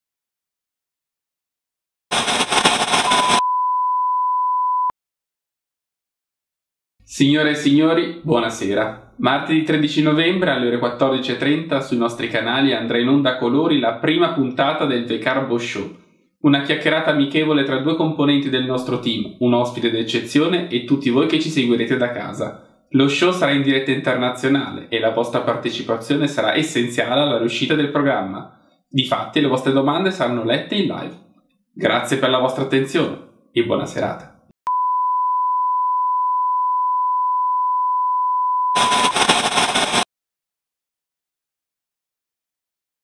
the CARBO Show! Annuncio Signorina Buonasera!
Avevamo bisogno di un'annunciatrice d'eccezione per comunicarvi che martedì 13/11 alle 14:30 andrà in onda in diretta la prima puntata del the CARBO Show! Un ringraziamento speciale alla nostra splendida Signorina Buonasera! Vieni a trovarci su twitch per la diretta!